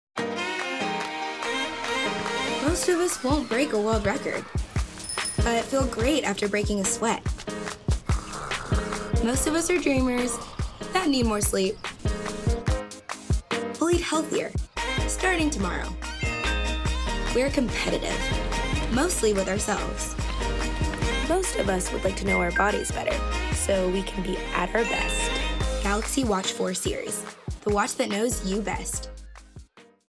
Galaxy Watch4: The watch that knows you best | Samsung
Most of us would like to know our bodies better, so we can be at our best. With Galaxy Watch4 series, track your health and wellness goals, jumpstart new routines and make positive changes everyday.